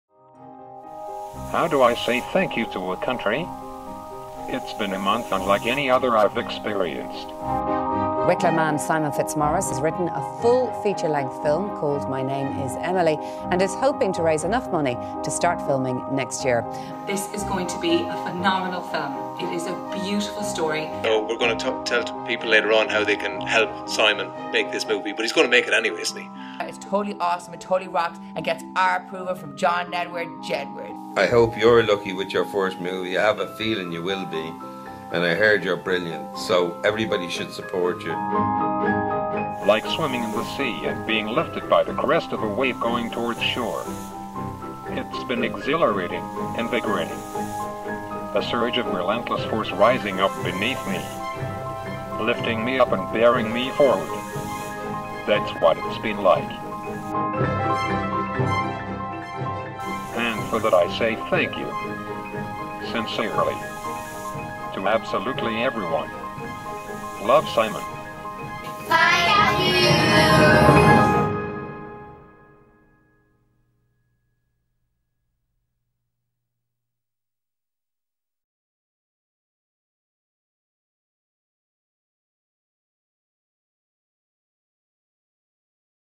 THANK YOU from Simon Fitzmaurice
MY NAME IS EMILY is a feature film by Simon Fitzmaurice, an award winning Irish film-maker with MND (ALS) Produced by Kennedy Films and Newgrange Pictures

Simon Fitzmaurice was diagnosed with Motor Neuron Disease in 2008 - just after his film THE SOUND OF PEOPLE screened at Robert Redford's Sundance Film festival.

At the time, Simon was the father of two children, his wife Ruth was pregnant with their third, and he had been heralded as "Ireland's most promising film-maker" by Film Ireland

But Simon refused to allow his disease to affect his drive towards becoming a feature film director. In the face of this adversity he wrote the script for MY NAME IS EMILY.

Help support the Crowdfunding campaign for MY NAME IS EMILY